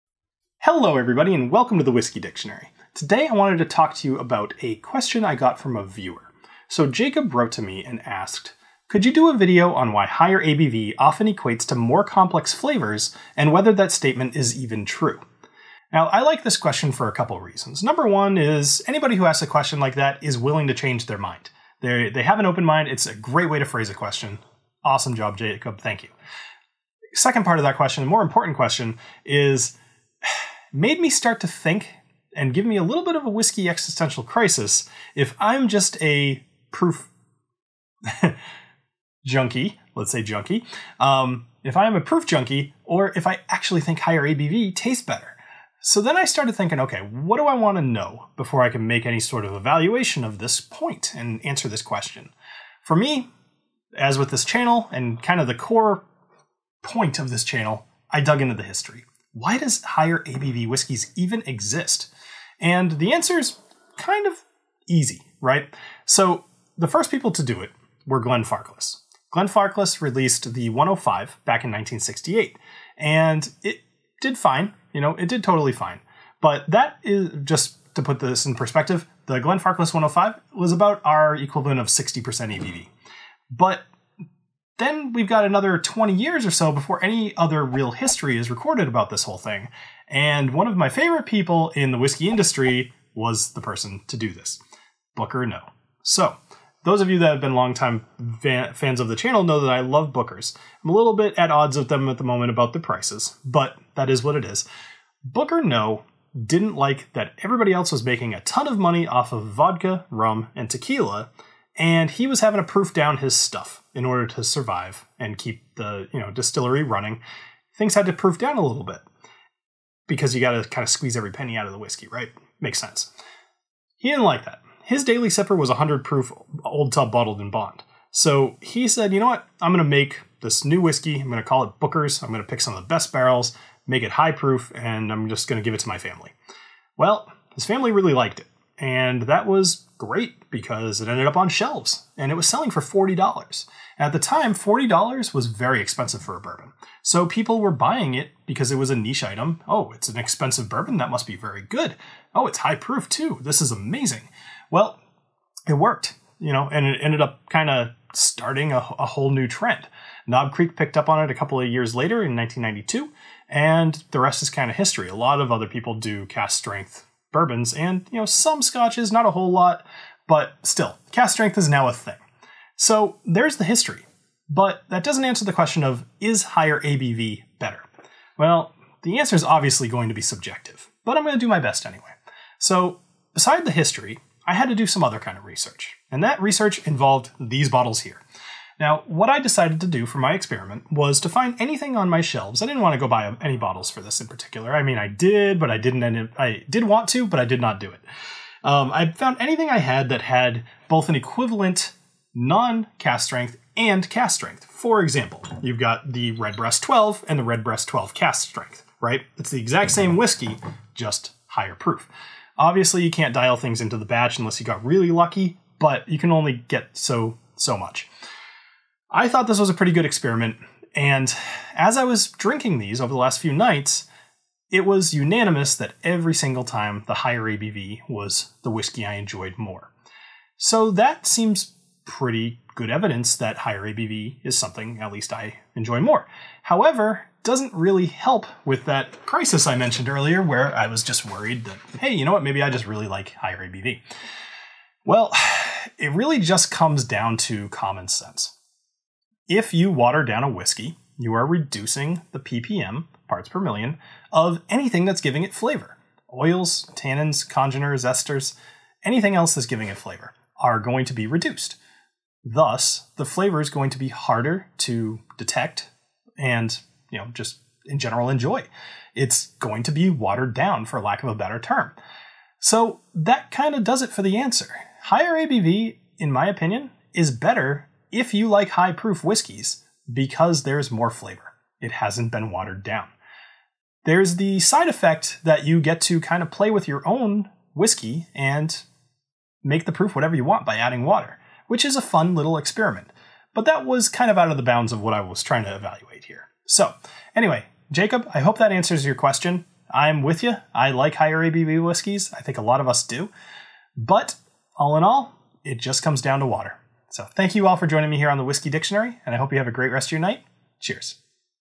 Is Whiskey Better at Cask Strength? | The Whiskey Dictionary
Ever wonder to yourself if you truly like higher ABV whiskies or if you've just been convinced that it's better?  Ever stopped to think about if ABVs are better?